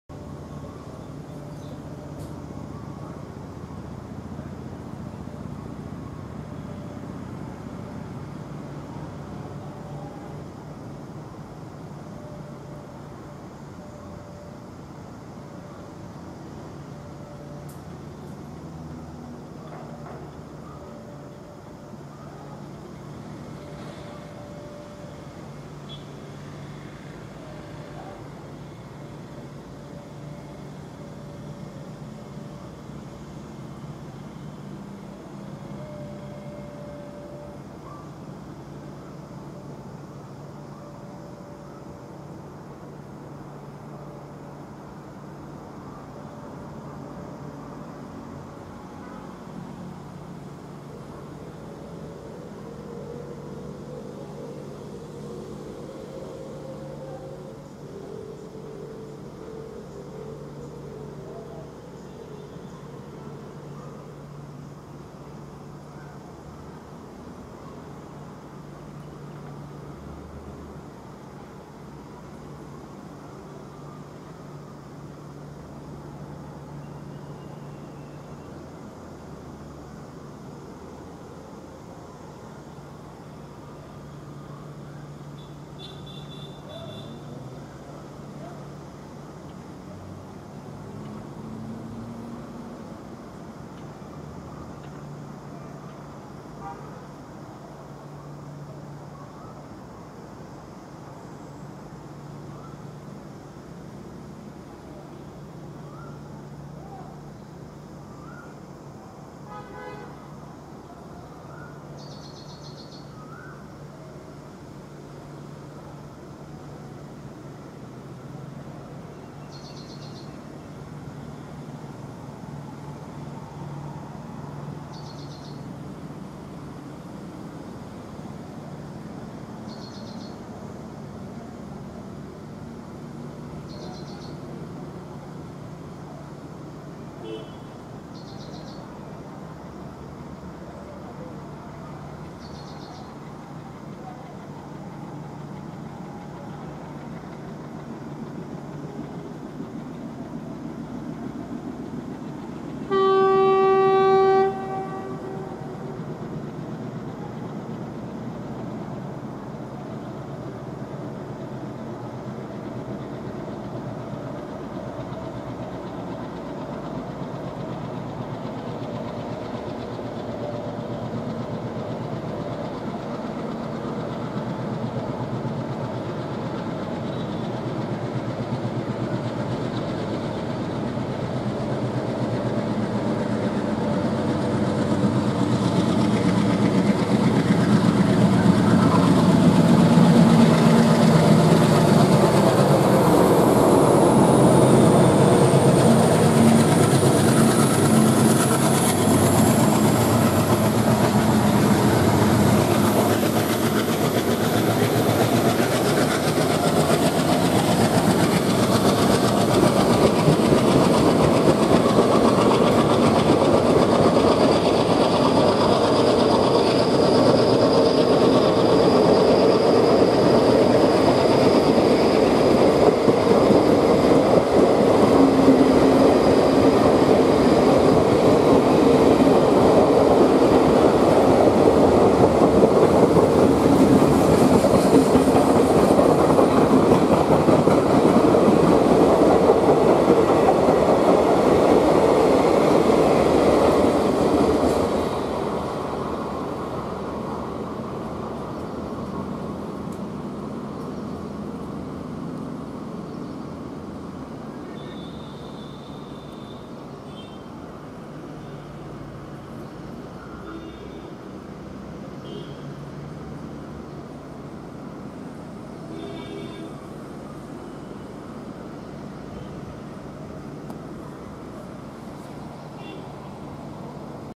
12628 Karnataka Express 20160417 12:50
Arrives on time at Biayyappanahalli (BYPL)

Loco: 11180 WDM-3D GTL; 16387 WDM-3A KZJ
Train; 12628 Karanataka Express
Location; BYPL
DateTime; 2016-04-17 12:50
Camera; Panasonic V270